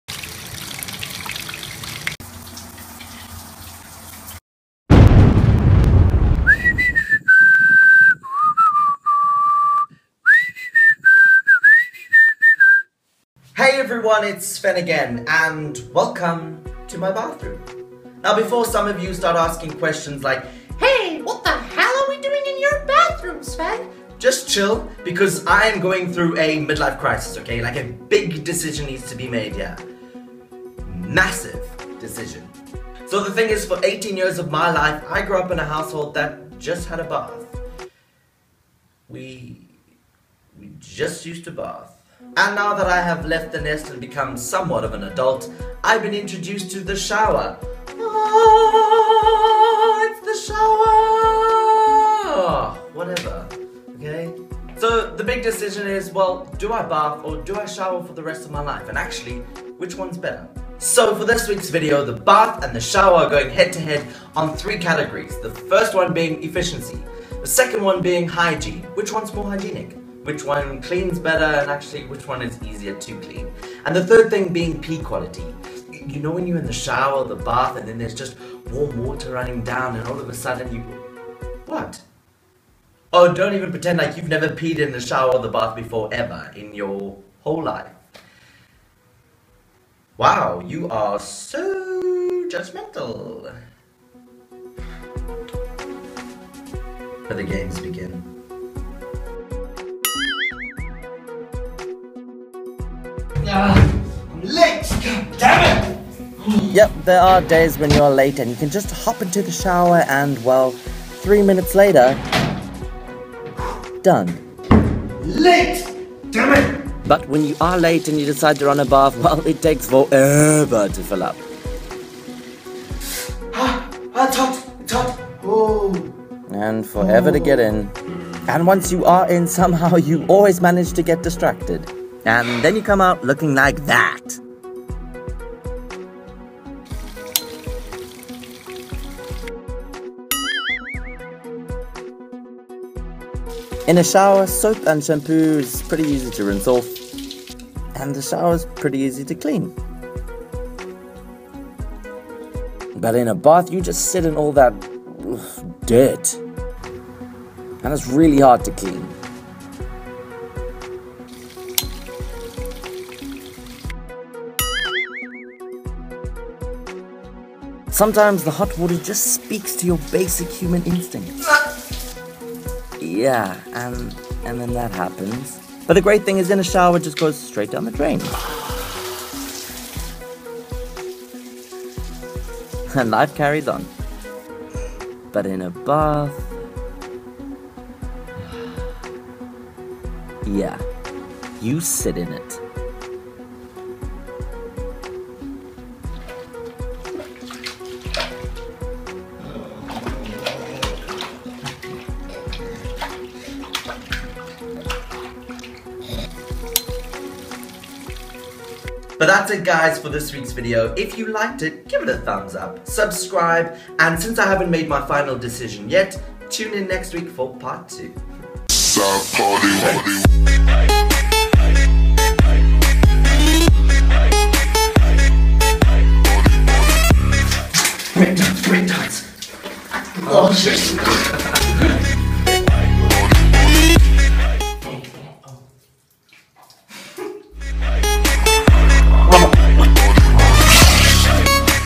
BIG DECISION !!! - Part 1
Its the ultimate battle between the BATH & the SHOWER

Two competitors, each with their own unique specialities but there can only be ONE WINNER!

WHO WILL IT BE...?